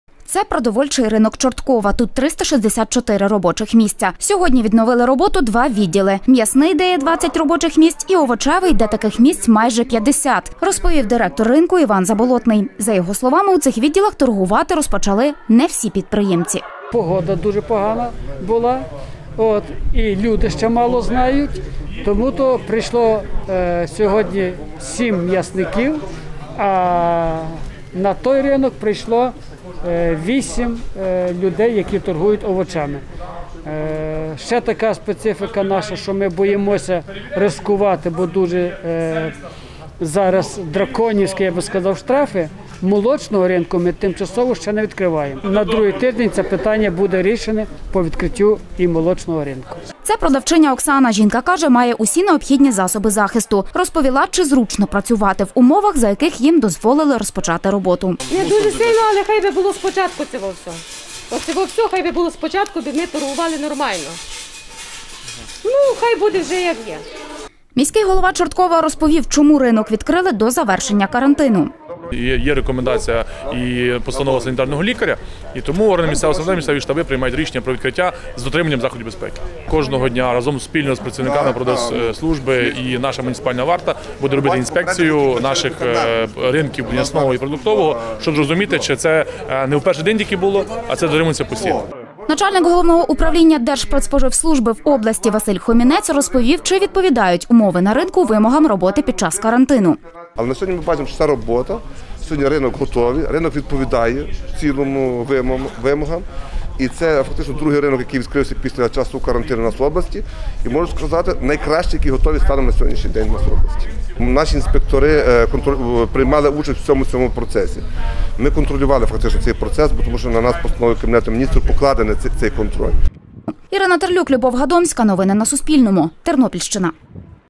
Запрацював продовольчий міський ринок Чорткова
Частково запрацював продовольчий міський ринок Чорткова. Про це повідомив міський голова Чорткова Володимир Шматько. За його словами, рішення відновити роботу ринку прийняв місцевий штаб для боротьби з коронавірусом.